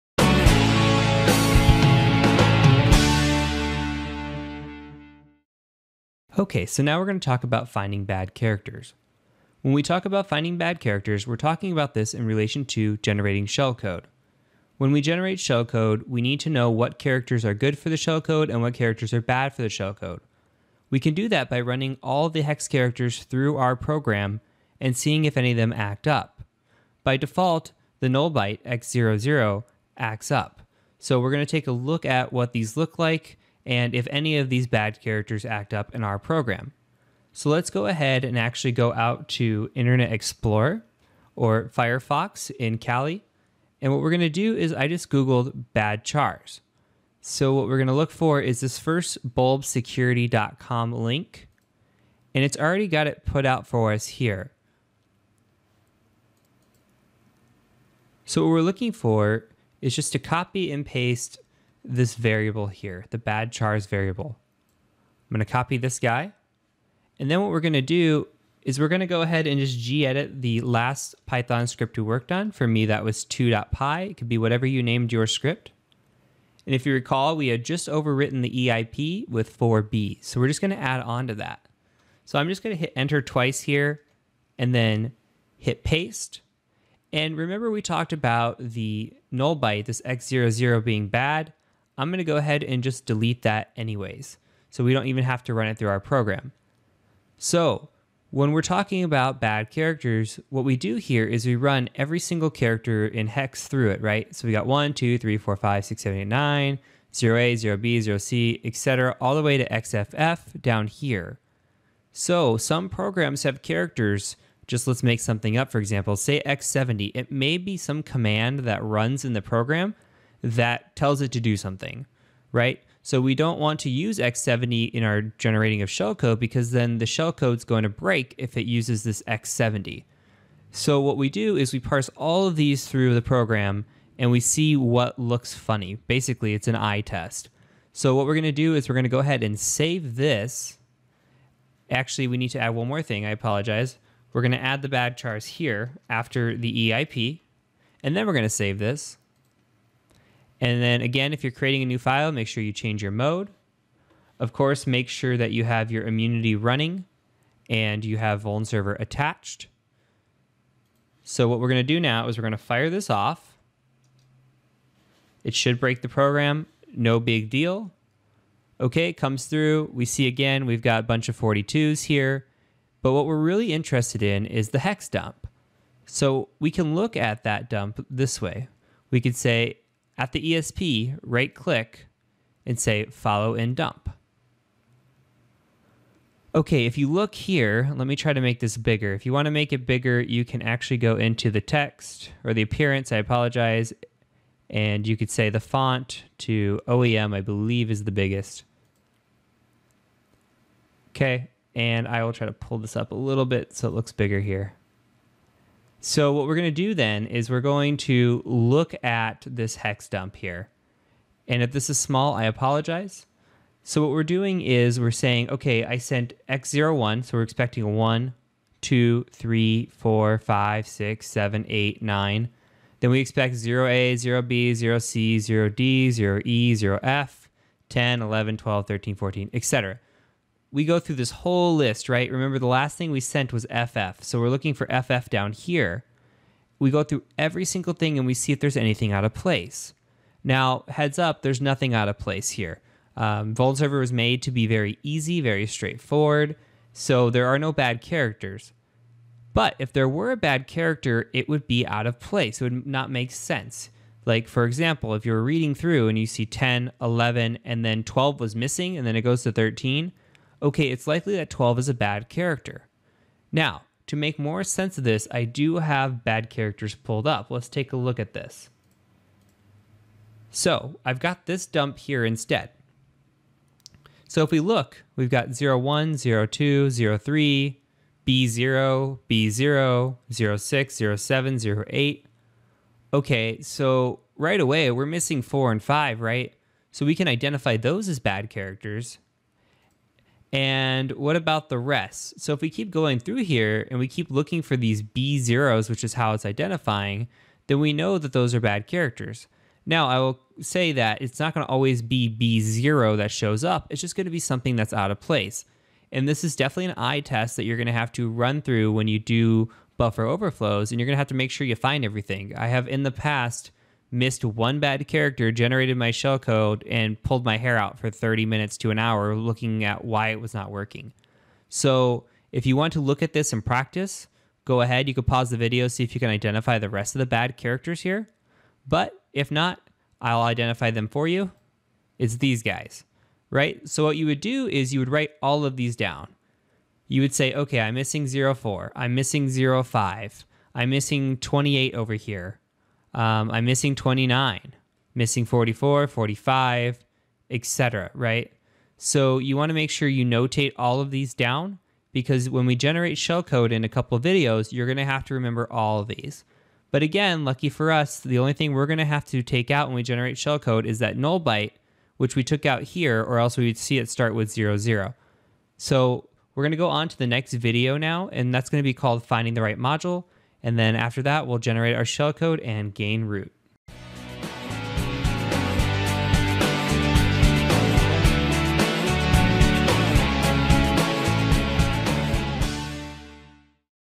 Buffer Overflows Made Easy - Part 6: Finding Bad Characters
This video covers how to find the find bad characters in a buffer overflow process.  We will examine the ESP dump and learn what bad characters look like, how they interact with shellcode, and their importance.

Timestamps:
0:05 - Introduction and setup
0:37 - Grabbing badchars list
2:40 - Executing script and reviewing results
4:29 - Second bad character dump review